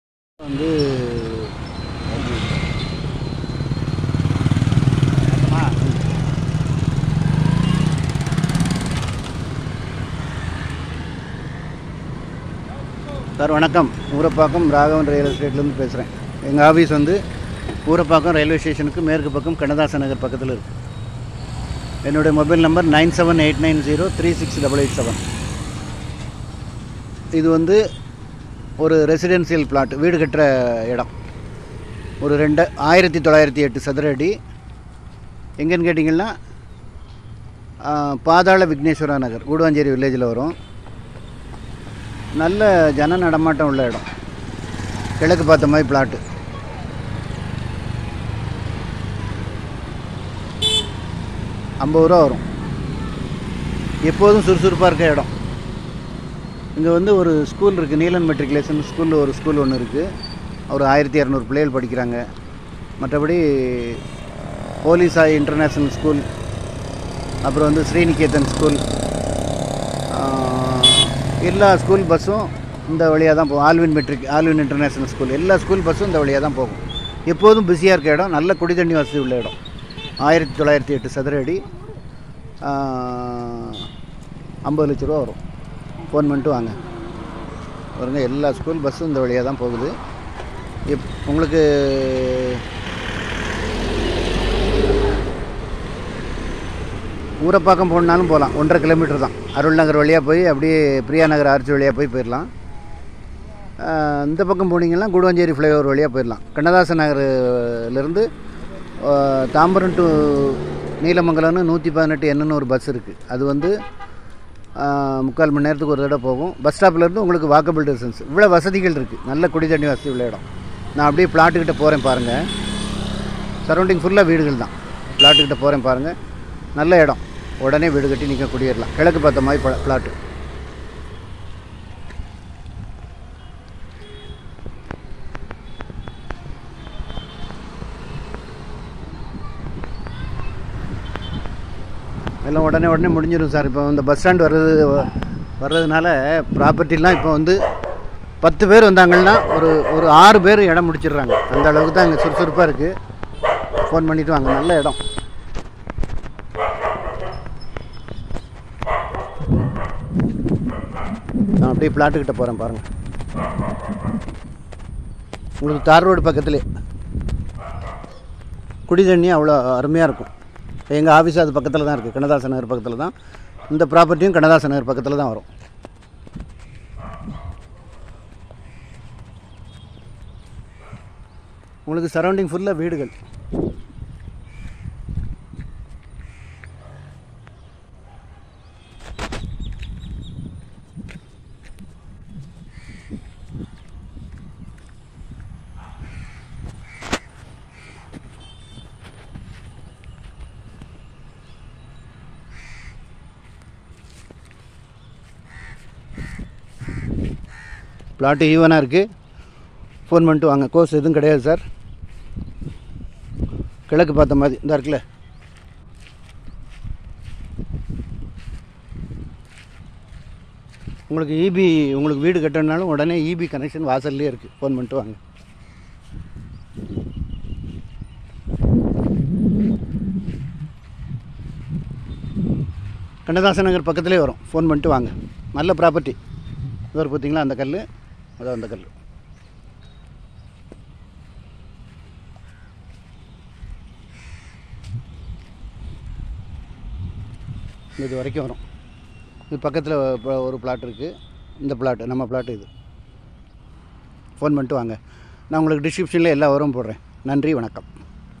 id no 449 Ragavan Real Estate urapakkam GST ROAD near Residential plot 1908 sqft east facing
Ragavan Real Estate at urapakkam west

 kannadasan nagar near Residential plot

Padhalavigneswara nagar 1908 sqft east facing

on 24 feet road Rate 50 Lakhs,sweet ground

water,nearby neelan matriculation school, GST

ROAD near ,surrounded fully Residence area

all school buses are came this way.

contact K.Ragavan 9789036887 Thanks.